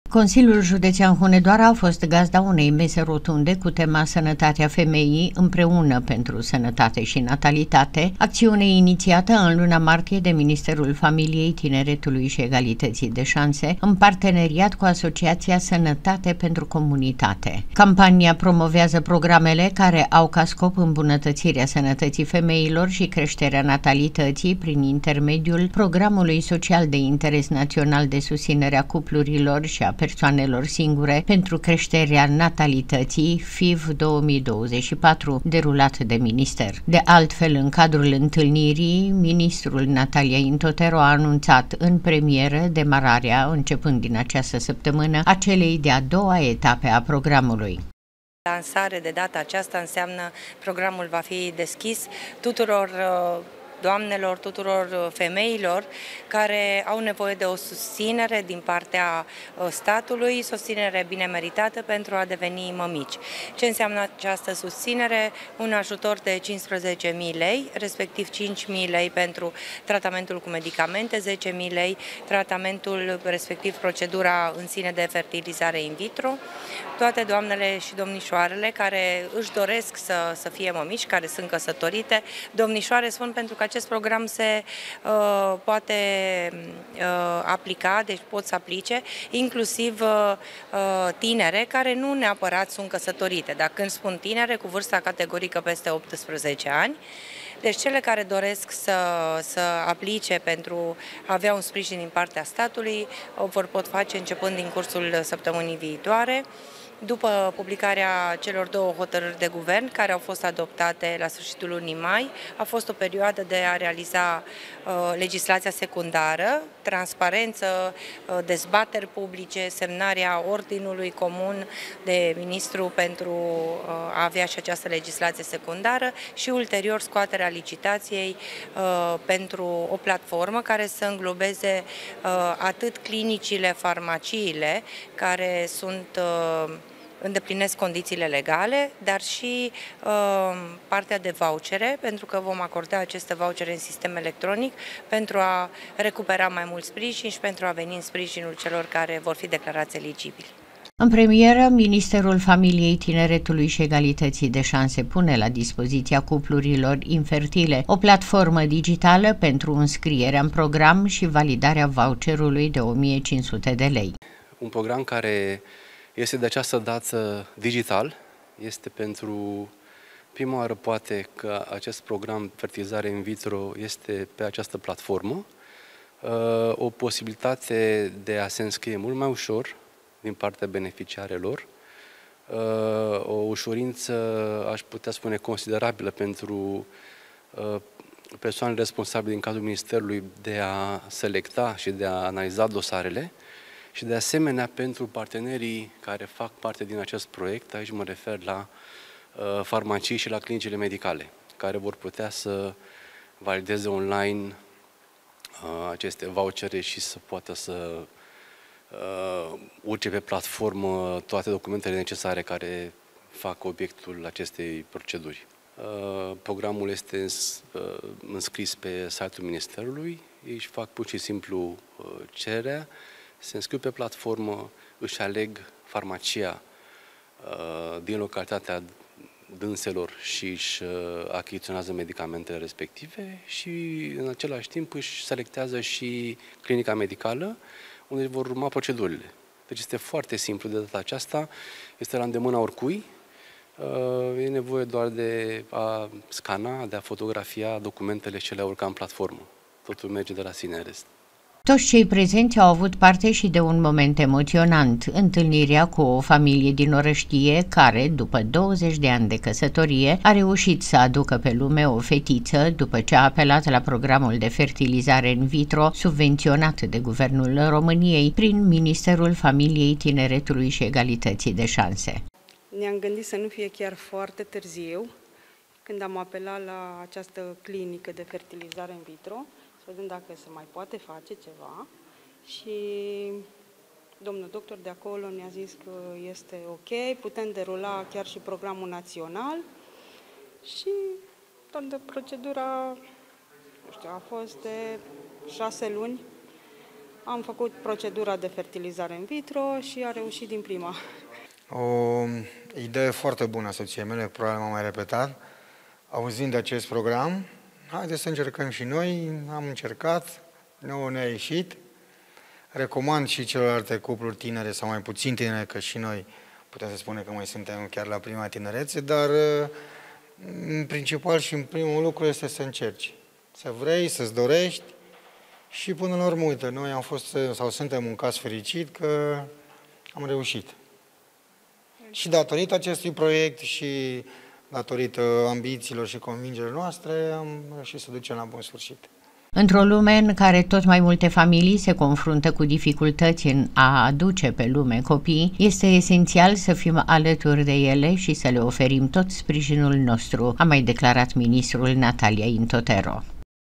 N. INTOTERO: „ÎMPREUNĂ PENTRU SĂNĂTATE ȘI NATALITATE !”
C.J. HUNEDOARA, GAZDA CAMPANIEI DE PROMOVARE A PROGRAMULUI „FIV 2024” INIȚIAT DE MINISTERUL FAMILIEI